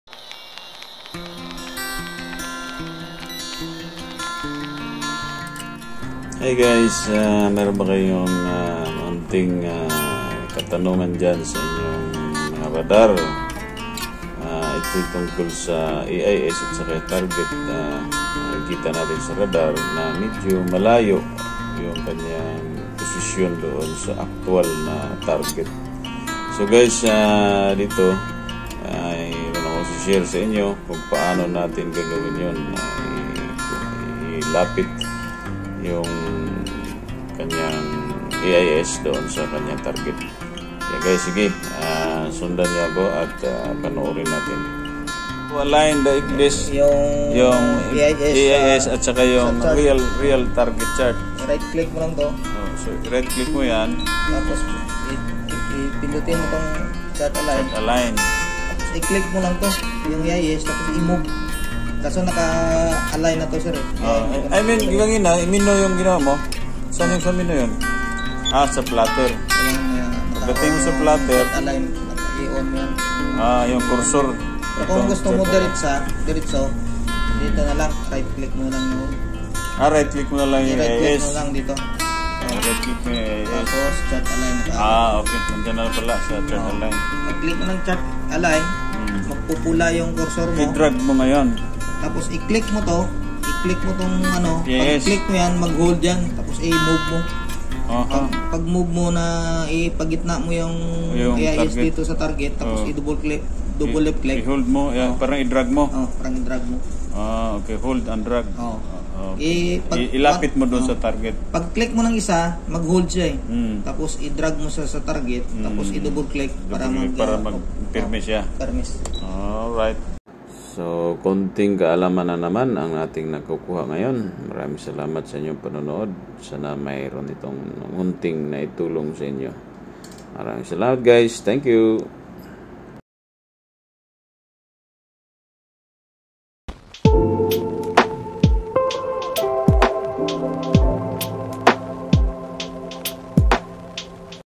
HOW TO ALIGN AIS POSITION AND THE ACTUAL TARGET ON ARPA RADAR
aligning the true position of radar target and ais. (automatic identification system)